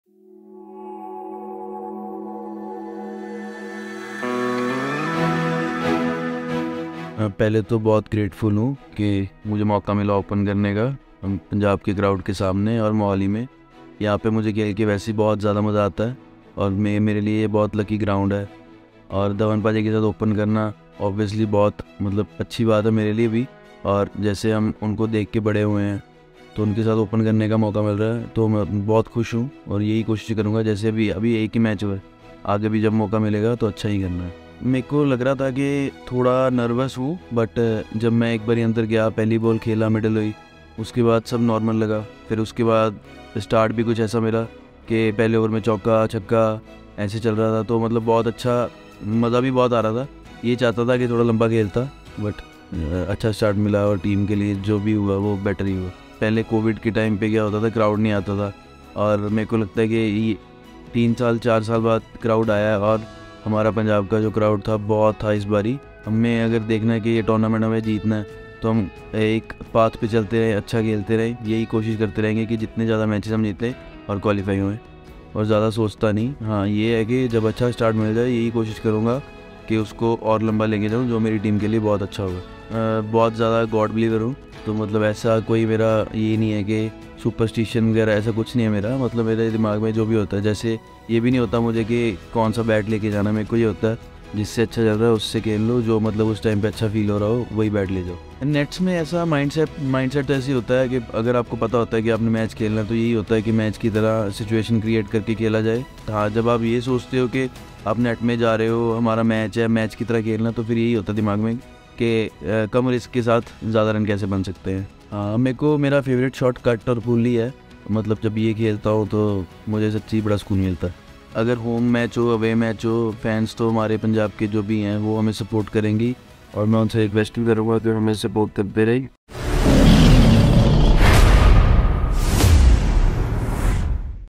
Prabhsimran Singh Reflects on His Opening Match Exploits
"It was fun for me to be out there." 🤩

Sadda 🦁 Prabhsimran Singh reflects on his opening match exploits and what we can expect going forward 💥